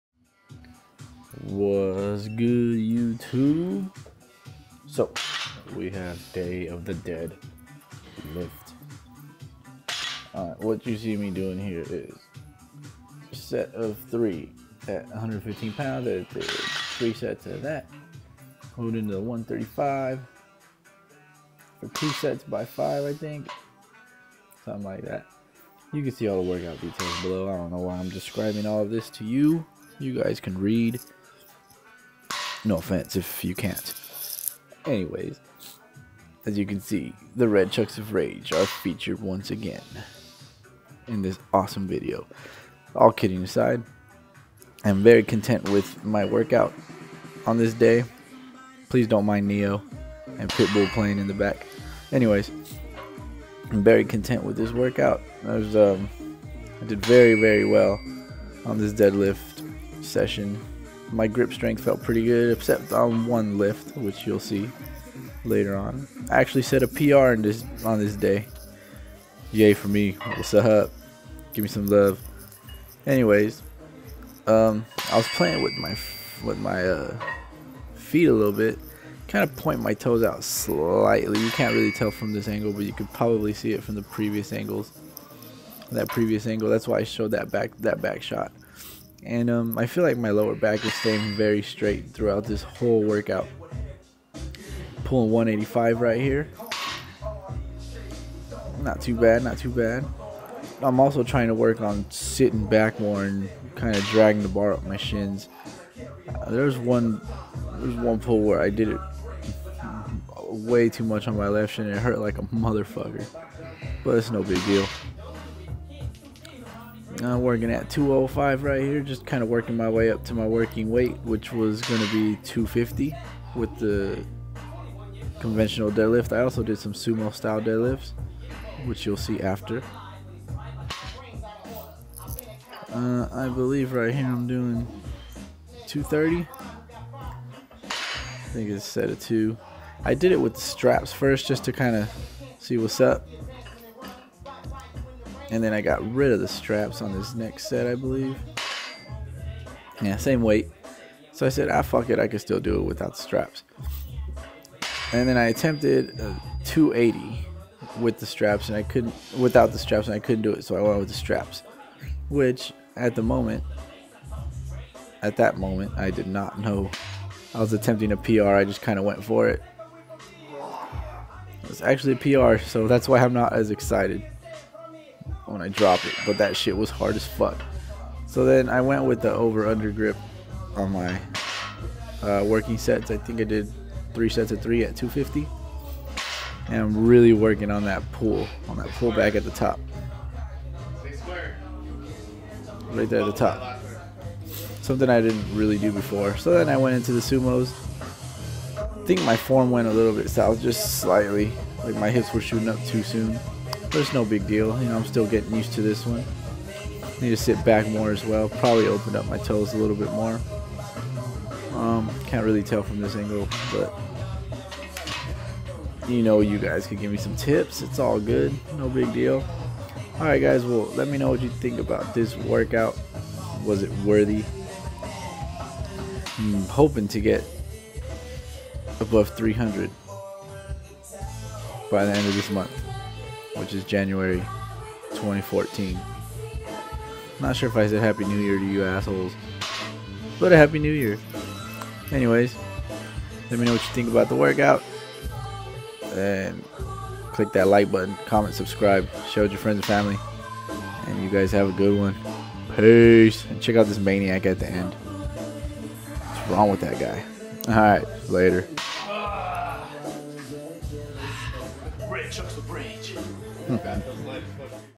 Day of the Dead ... Lifts
Here are the workout details:

Conventional Deadlift
95 x 5
115 x 3 x 3
135 x 5 x 2
175 x 3
185 x 3 x 2
205 x 2
230 x 2 x 2
280 x 1 (PR!)
250 x 3 x 3

Sumo Deadlift
185 x 5 x 3